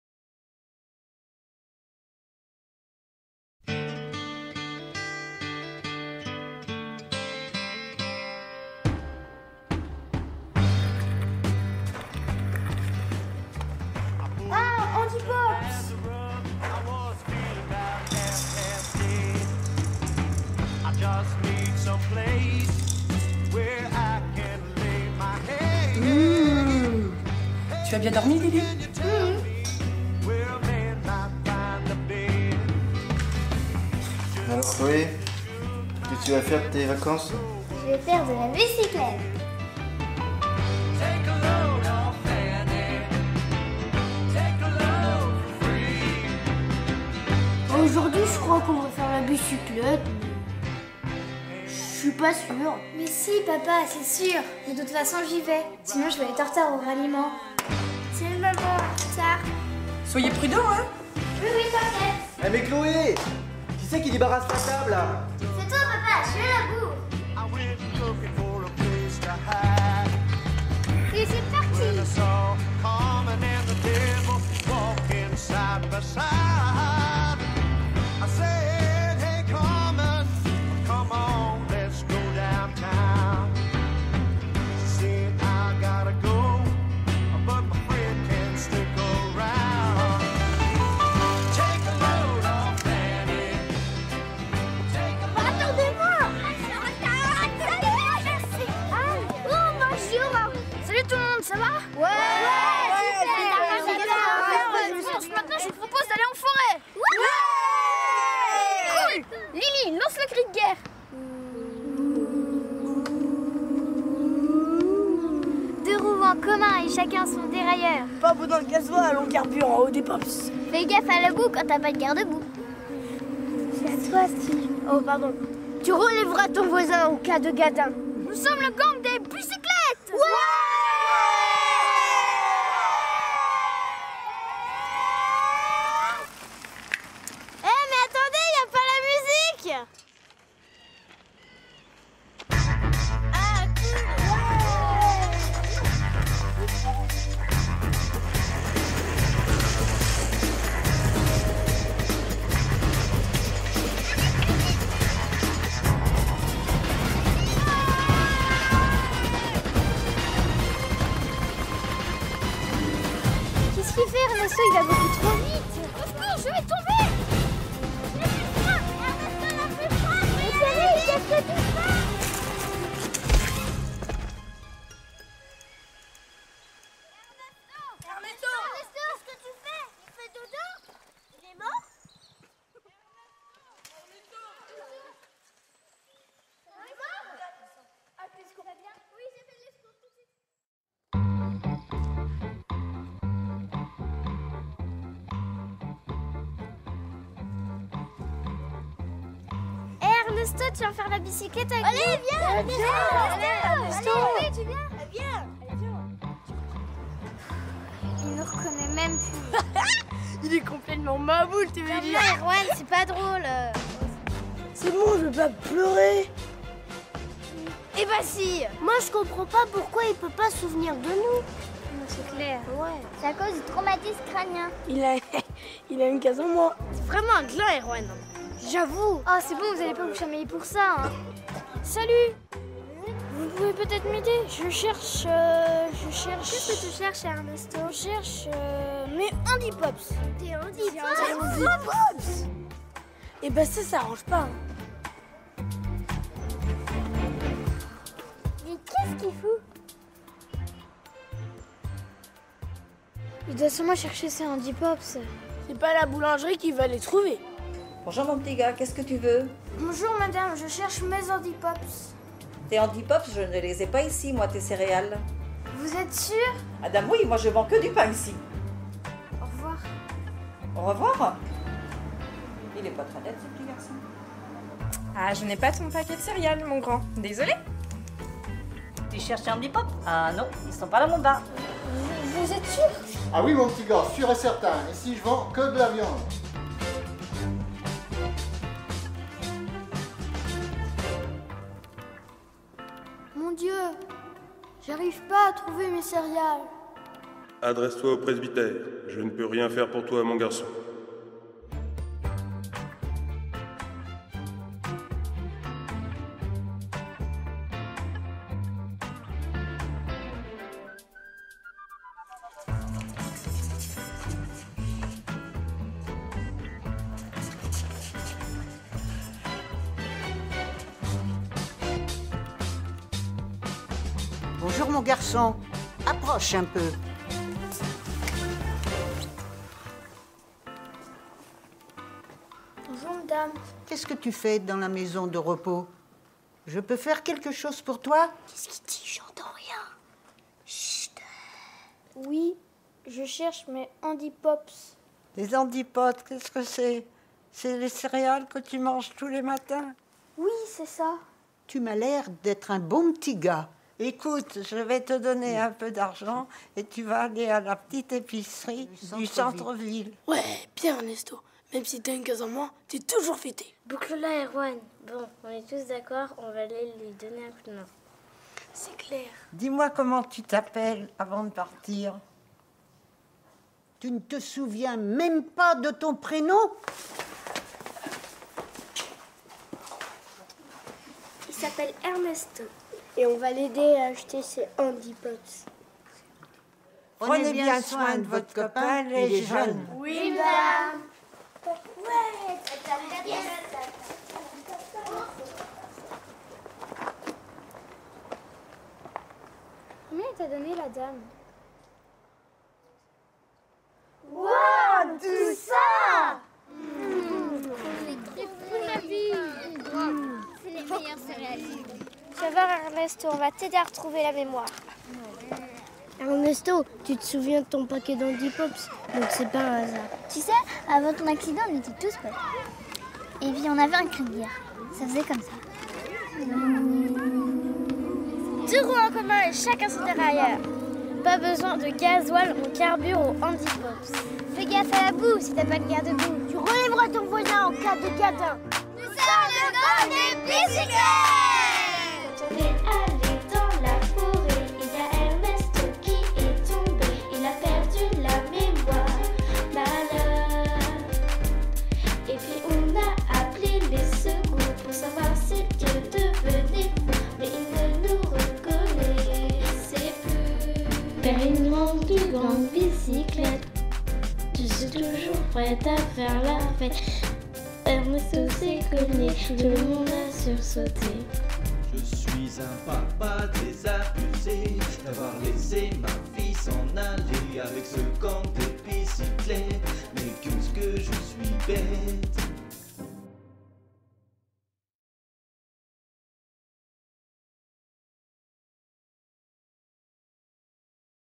ALJ PROD 2014 03 HANDIPOPS
Vidéo réalisée par les juniors de l'ALJ dans le cadre du Festiv halles de Printemps porté par le Service Culturel de la Ville de Savenay et dans le cadre du Festival Handiversité.